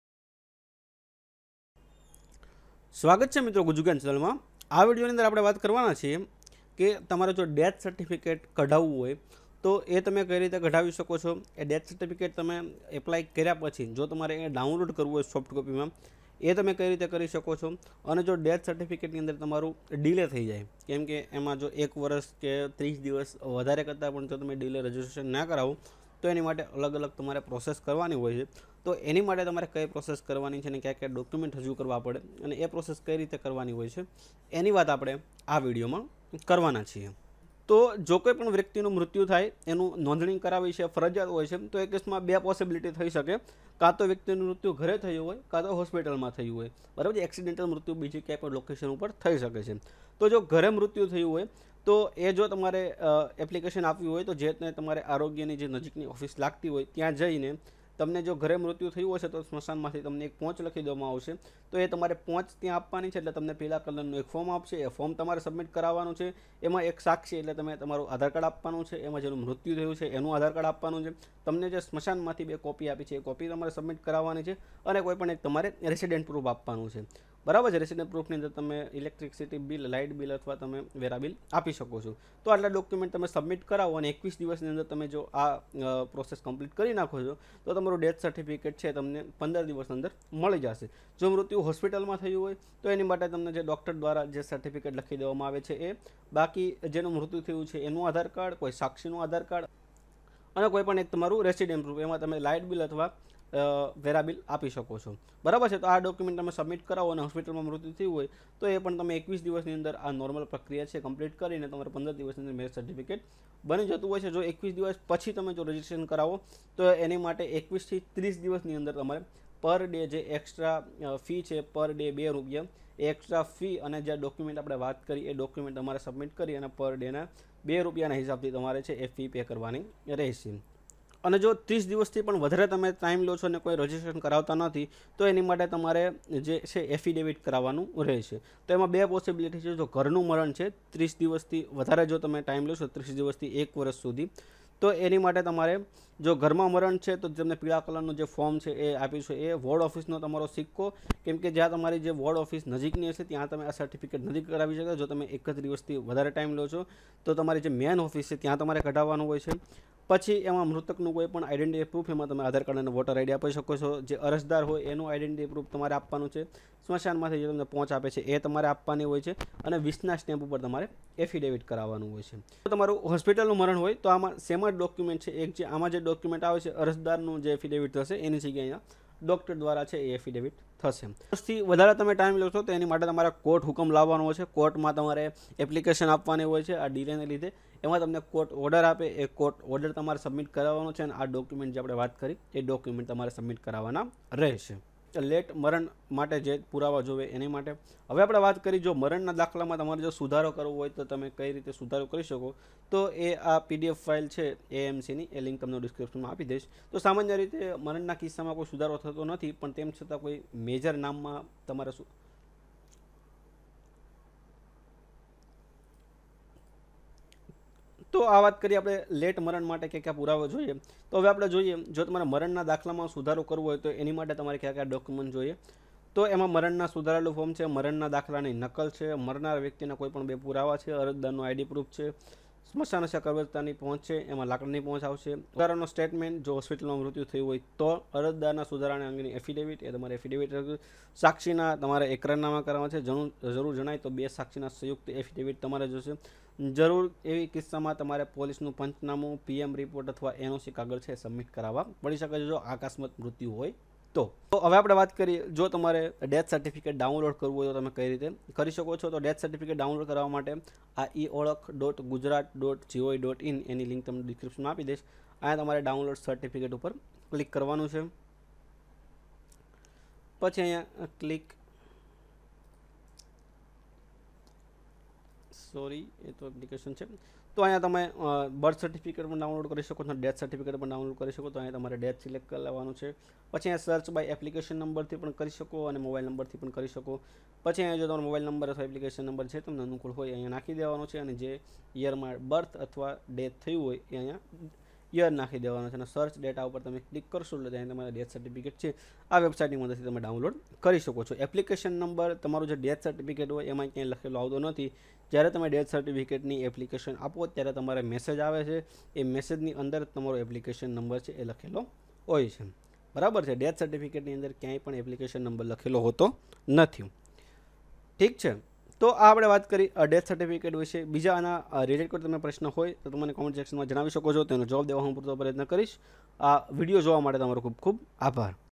Death Certificate Process Gujarat | Maran Nodhani Gujarat | Late Death Certificate Gujarat
Jay hind, aa video ma apade vat karvana chiye death certifcate yetale ke metan nodhani kai rite karvi sako tane death cerficate kai rite kadhavi sako yeni vat apade aa video ma karvana chiye